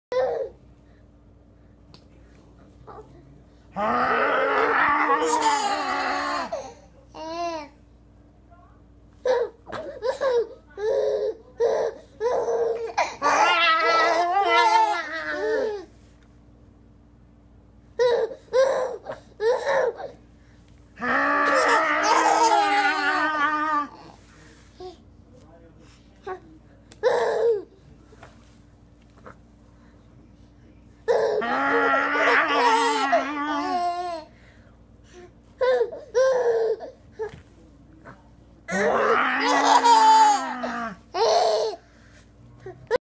Me and Daddy playing in the kitchen
I crawled all the way to the kitchen! I'm getting really good at crawling. I am using my arms so much that I look like the Hulk now! If only I could turn green.